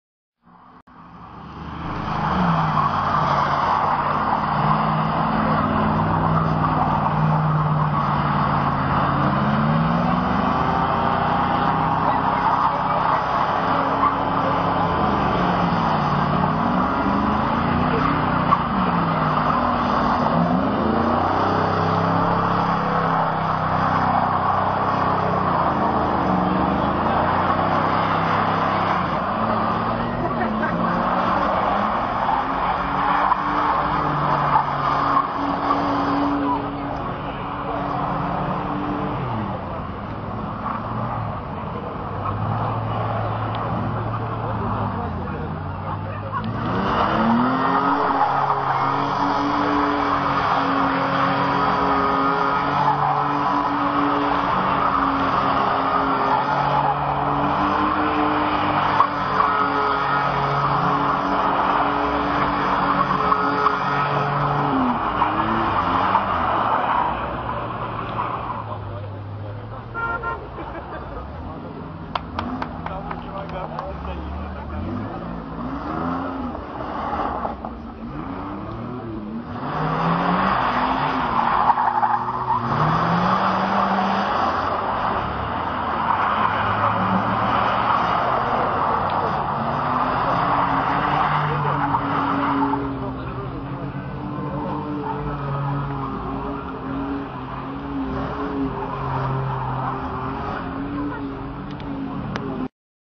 Muscle car отжигают.Muscle car burn rubber
Muscle car жгут резину на Автострада 2013 в Калуге.
muscle car drifting. Масл кар.